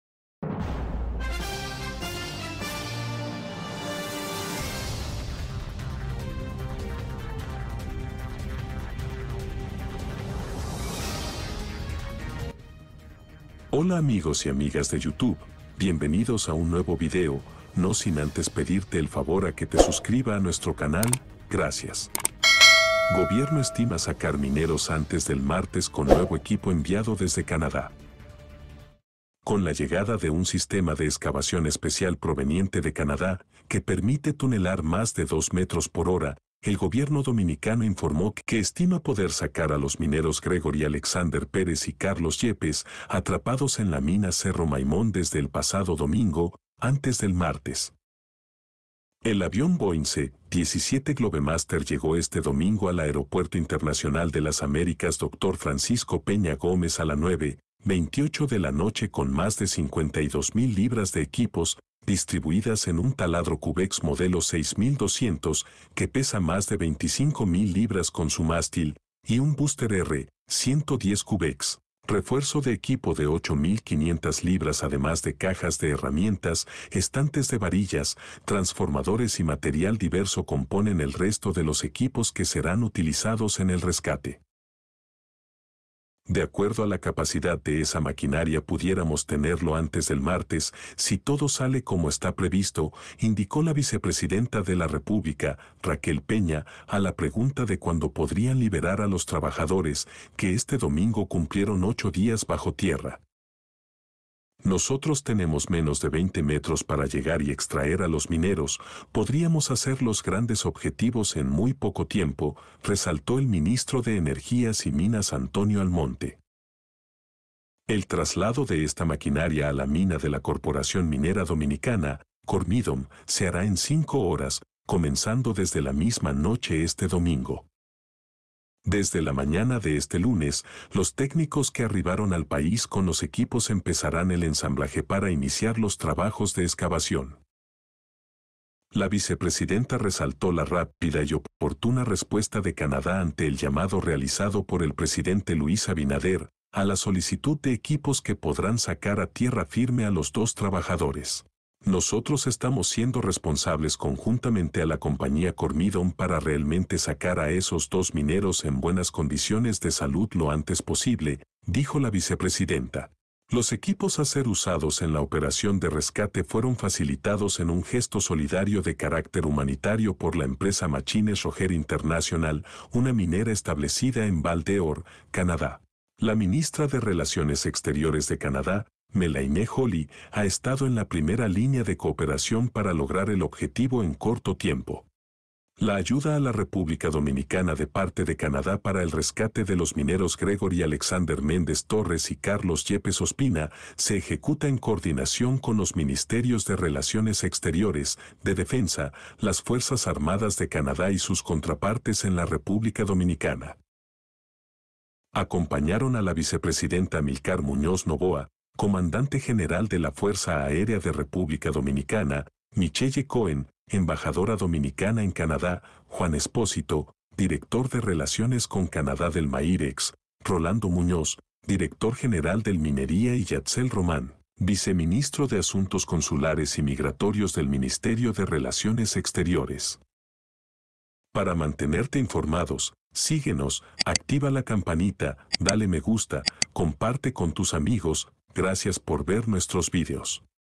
BUENAS NOTICIAS PARA MINEROS ATRAPADO EN MINAS DOMINICANA
Con la llegada de un sistema de excavación especial proveniente de Canadá, que permite tunelar más de dos metros por hora, el gobierno dominicano informó que estima poder sacar a los mineros Gregory Alexander Pérez y Carlos Yepes, atrapados en la mina Cerro Maimón desde el pasado domingo, antes del martes